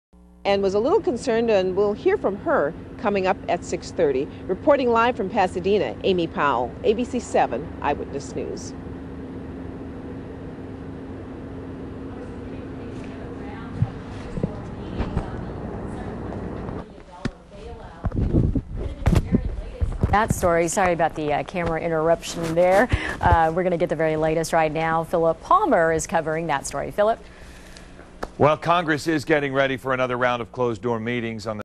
KABC 7 Camera Blooper!
Big time Los Angeles television!!  Look at the anchor dude ducking in the back!!!